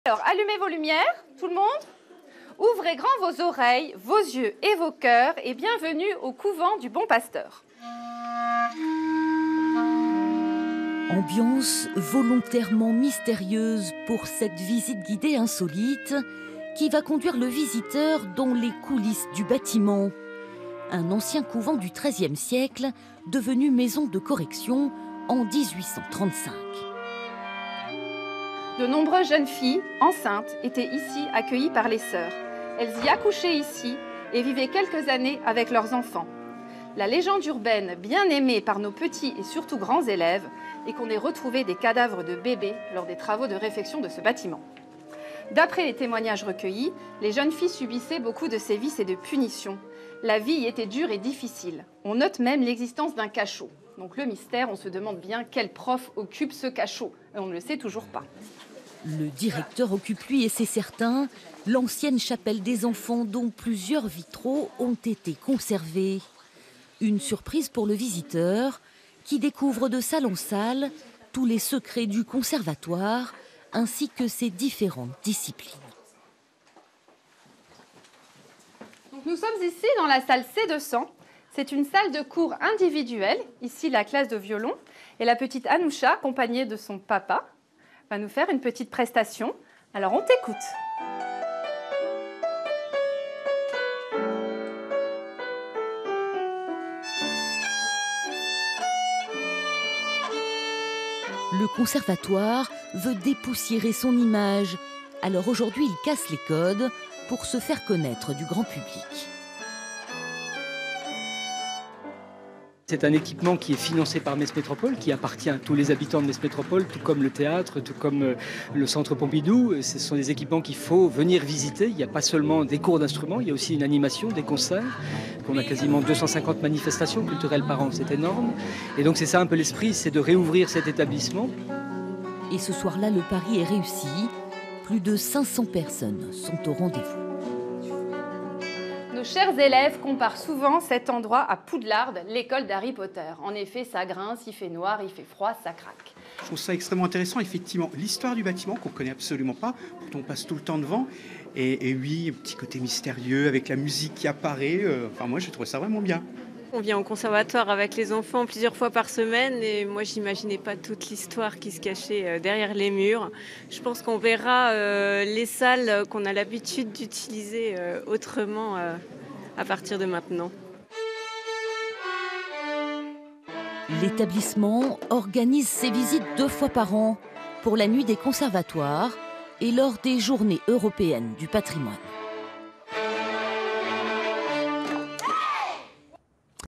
La nuit des conservatoires : visite des coulisses et des mystères du conservatoire de Metz
Dans le cadre de la nuit des conservatoire, 500 visiteurs sont venus écouter les l'histoire et les élèves du conservatoire de Metz : au menu mystères et légendes, musiques et chants.
Reportage Séverine dangin - Guillaume Robin